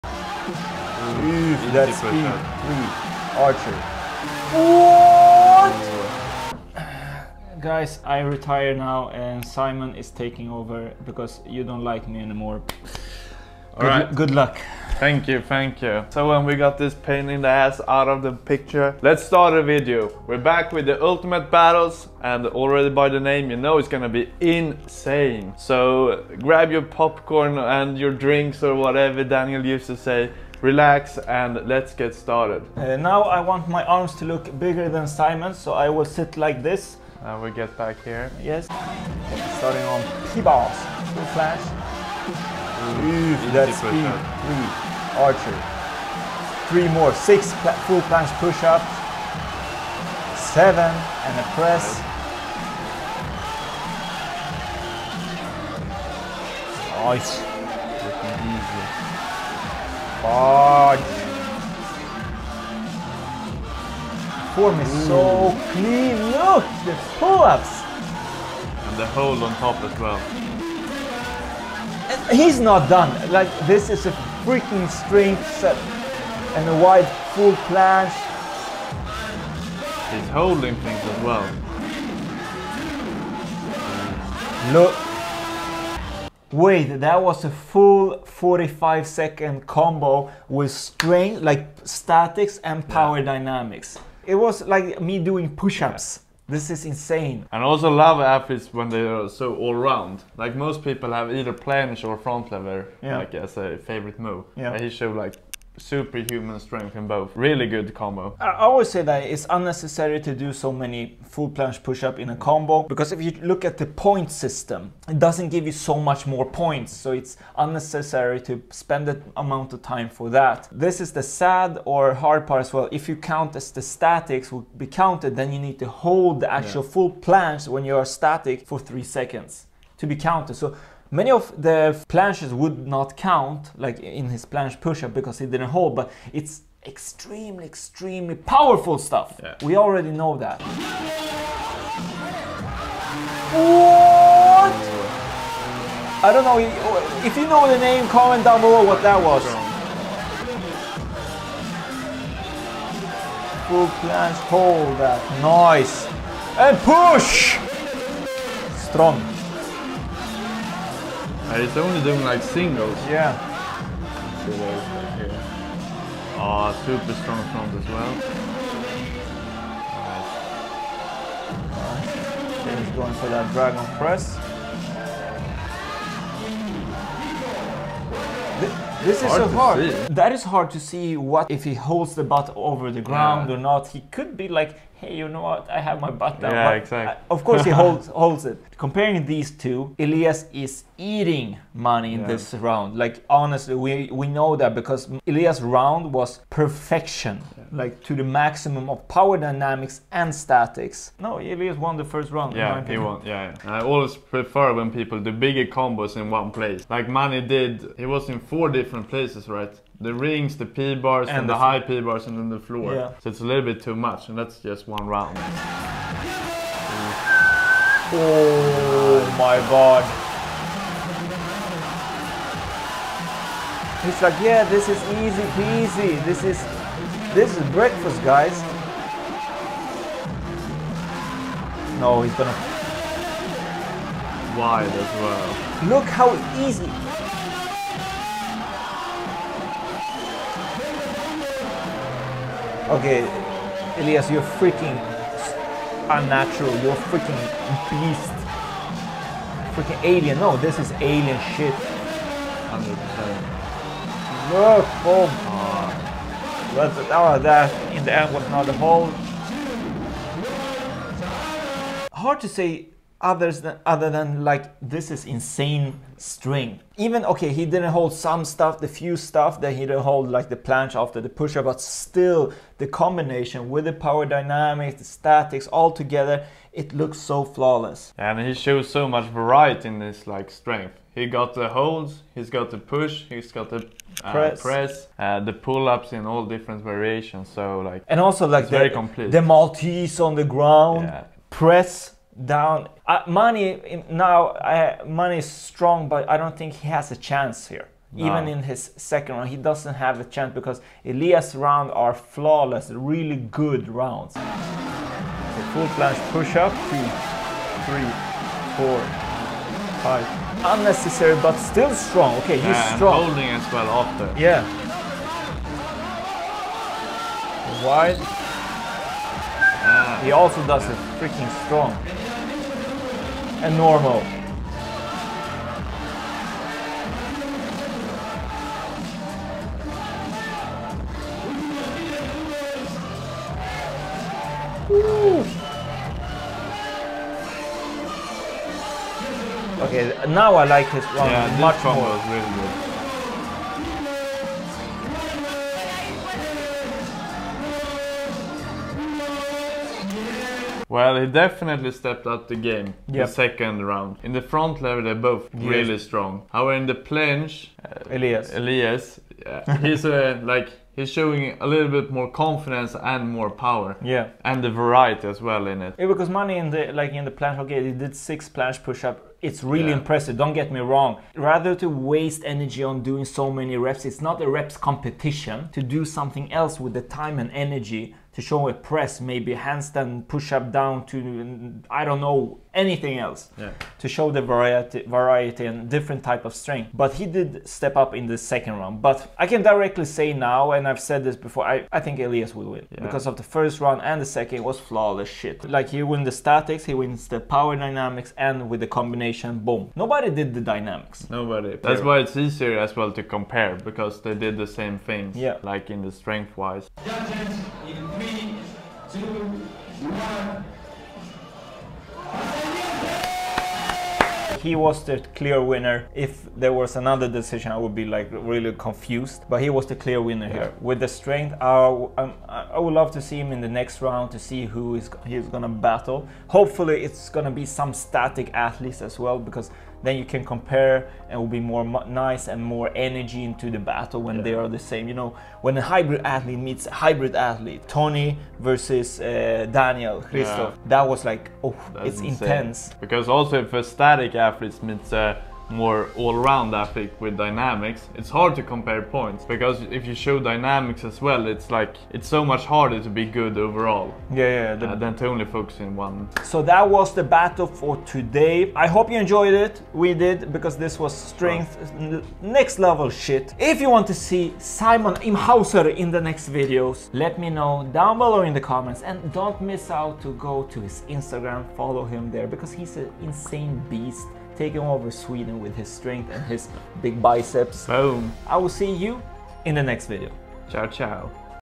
Whatever He Eats, I Want it - Iliesse Vs Mani
Street Workout Ultimate Battles this time Iliesse OZT against Mani Workout.

All interviews can be found in podcast format, you find it where you find all podcasts.